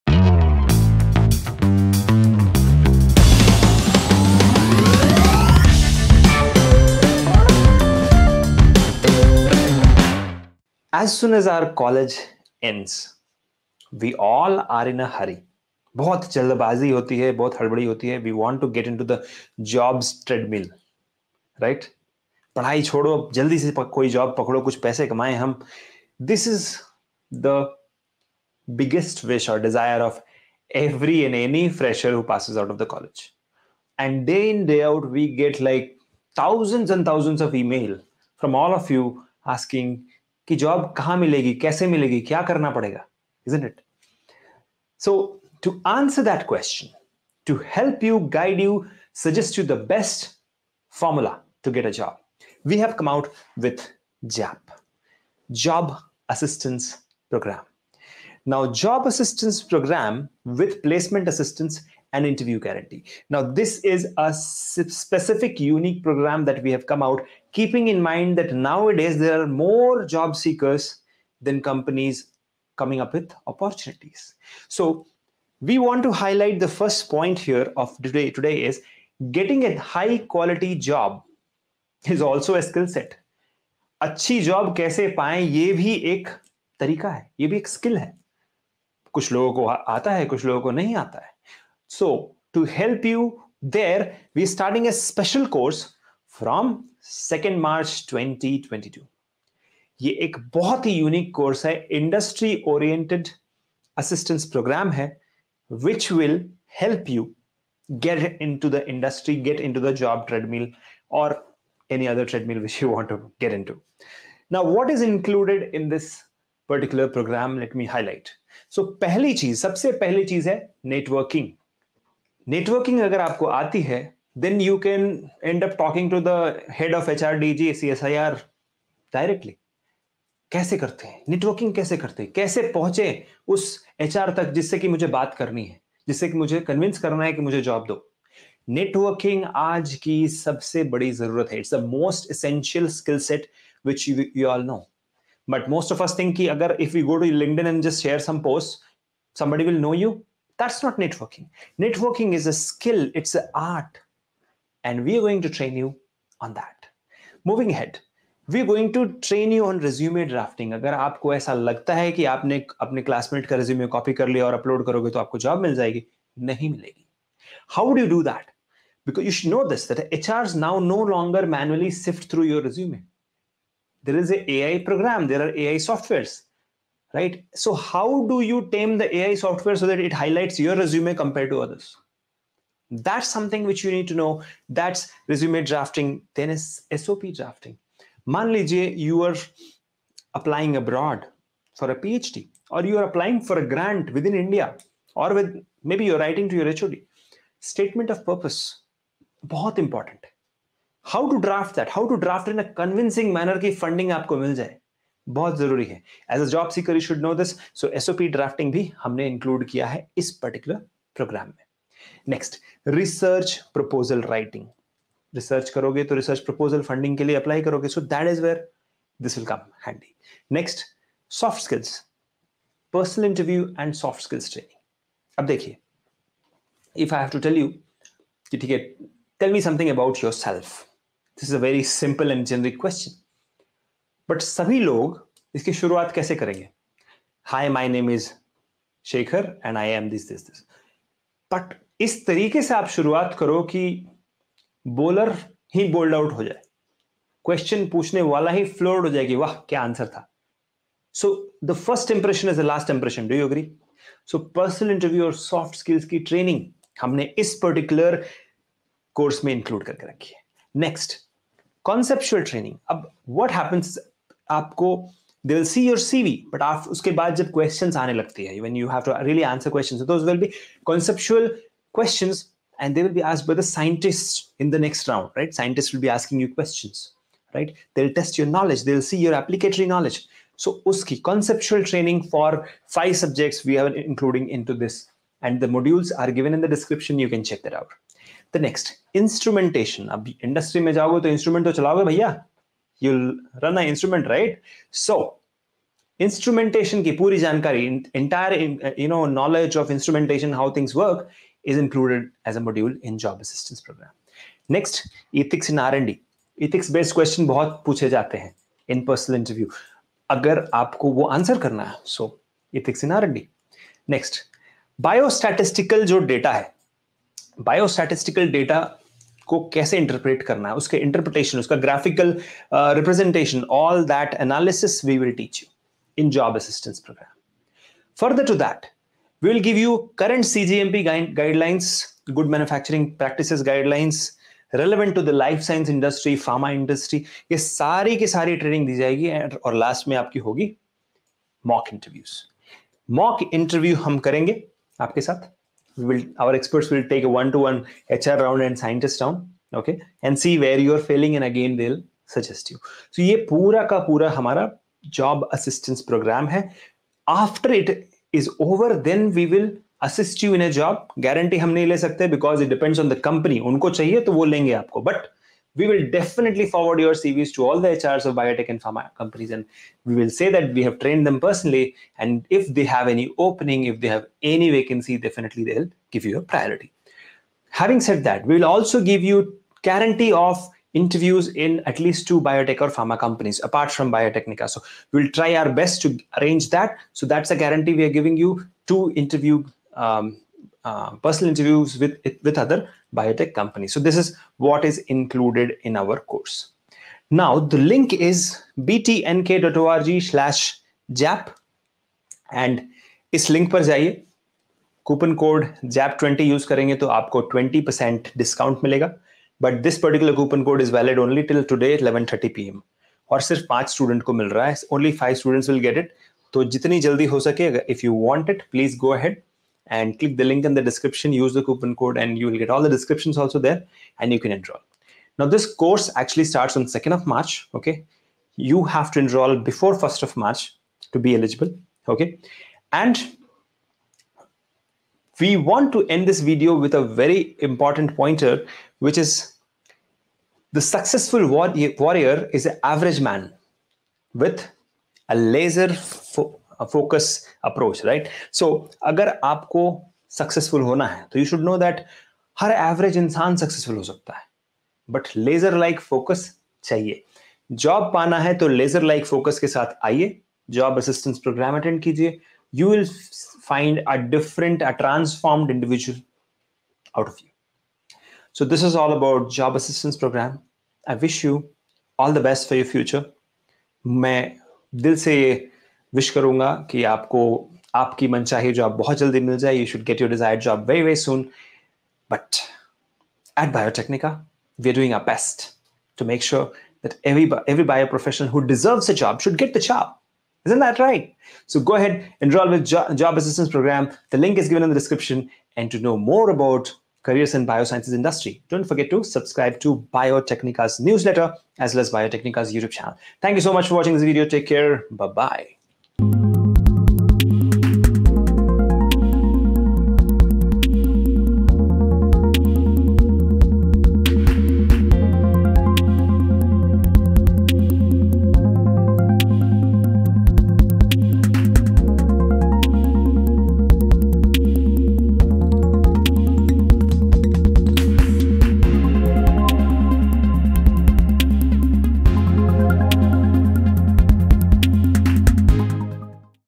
Biotecnika Launches Job Assistance Program (JAP) With Interview Guarantee
Register for Biotecnika 's Job Assistance Program

00:00  Introduction
00:17  About Job Assistance Program
01:51  what's included